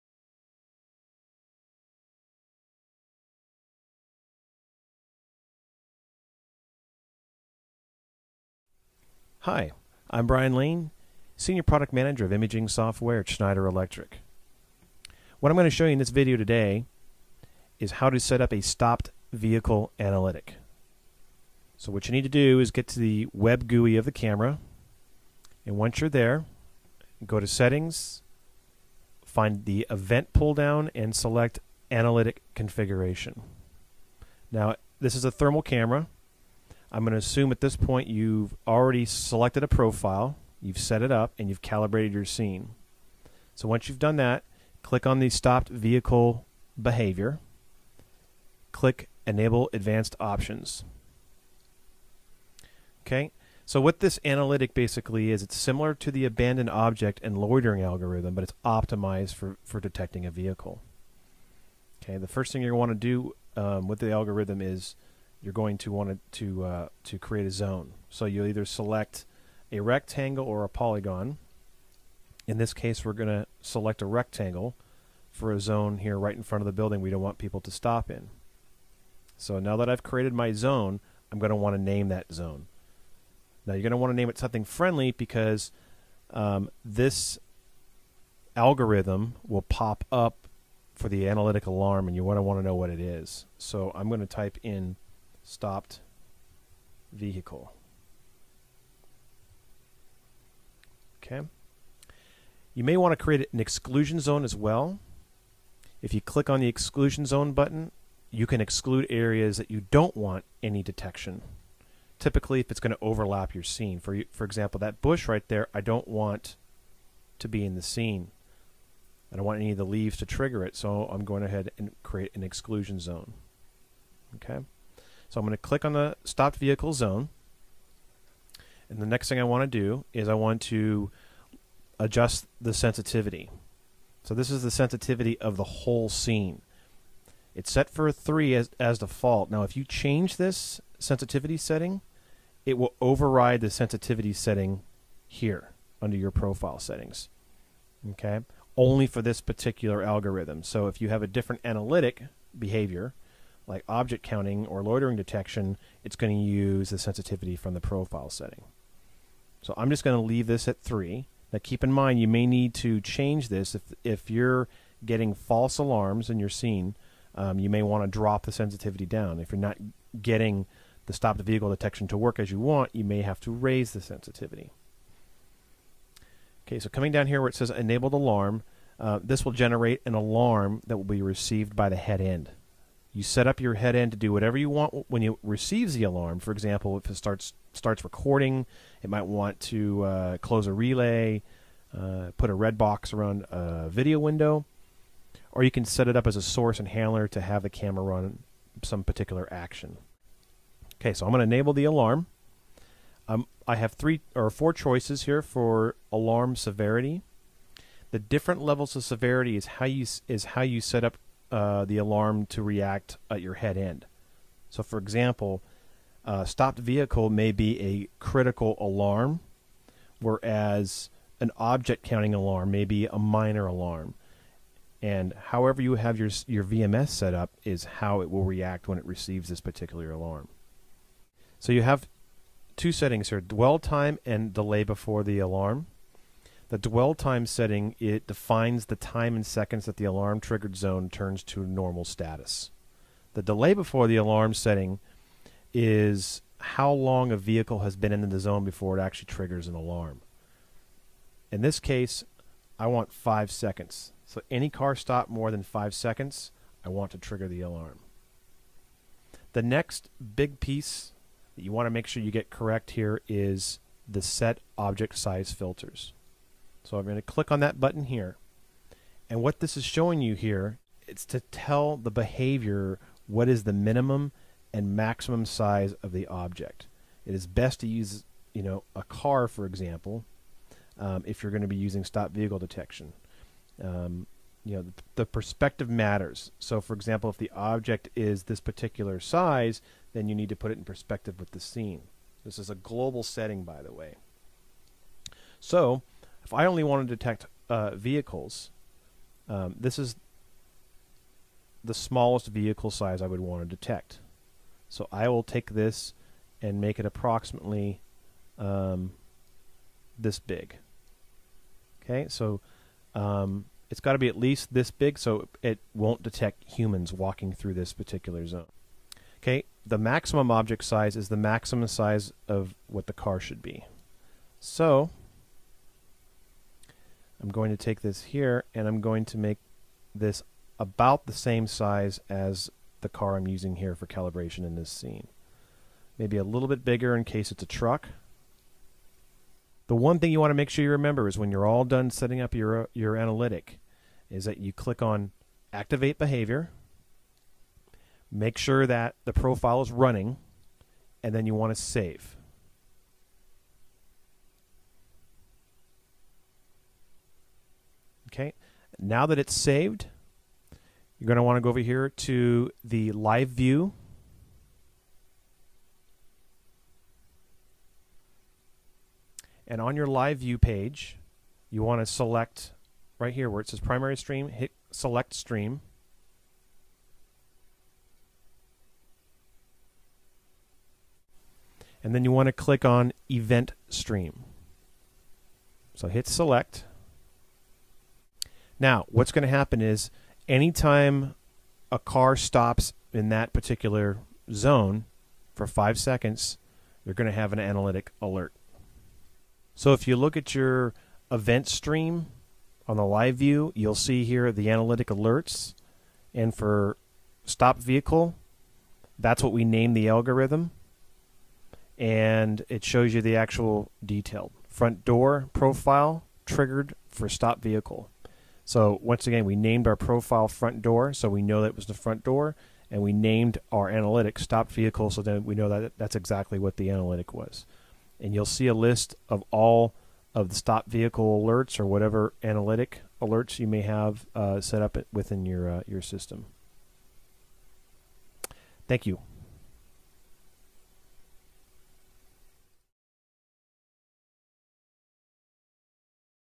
Pelco Analytics: Stopped Vehicle (1080p)
How to set up the Stopped Vehicle Detection Pelco analytic using a Sarix thermal camera.  NOTE: This video is cropped to comply with the 16x9, 1080p standard.  There is also a 4:3, 480p uncropped version of this video.